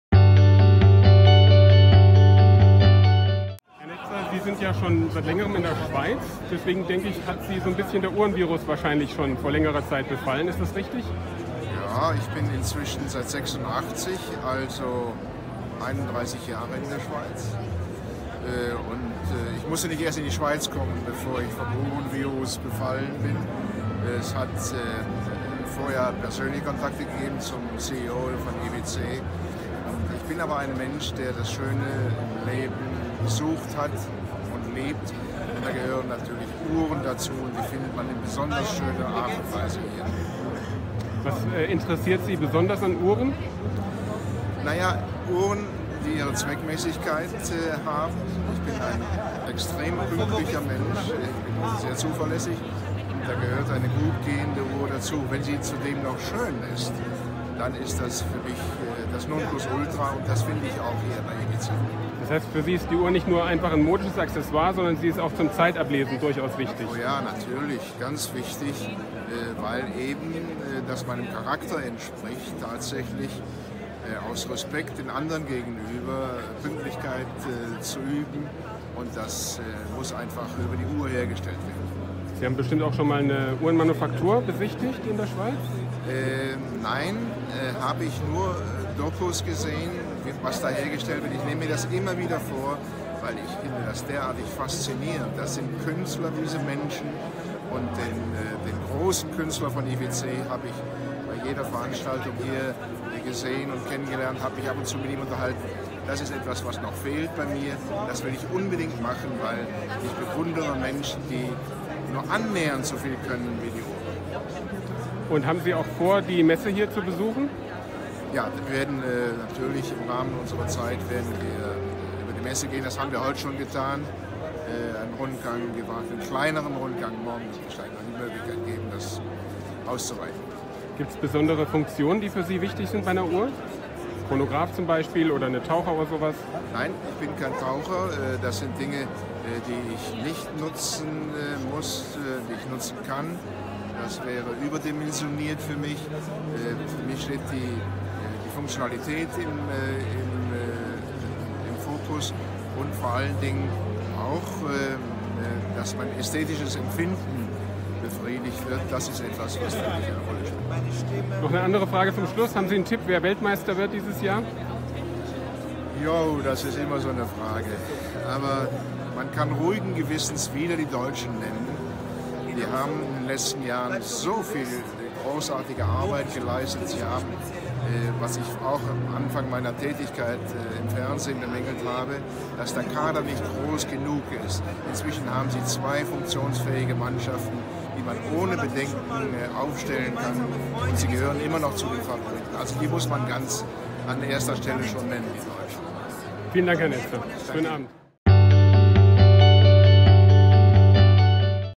Fußballlegende Günter Netzer auf der IWC-Gala
Auf der IWC-Gala während des Genfer Uhrensalons SIHH 2018 sprachen wir mit Günter Netzer, der zu den besten deutschen Fußballspielern aller Zeiten zählt und die deutsche Nationalmannschaft 1972 zum Europameistertitel führte. Im Videointerview verrät er, seit wann er schon Uhrenfan ist, was er an Uhren schätzt und ob er sich die Zeit genommen hat, sich spannende Luxusuhren auf dem Schweizer Uhrensalon SIHH anzuschauen.
[3862]